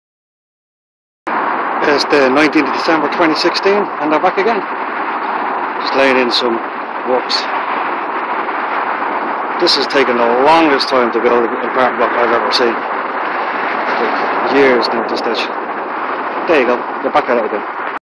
new bancroft 99
taken on the 19th of december 2016 almost a YEAR AND A HALF after the last vid and finally theyre back on this site building again. this is the longest build in tallaght ive seen by some stretch and as you'll see in the next two vids its still not finished. the site HAS been vulturefunded though (along with tallaght cross up by the square) which explains why building has started again as the new owners want to get it on market.

from this vid you can see work on the surrounds of the build with railing and brick work going on while concrete pouring starts on one of the central blocks.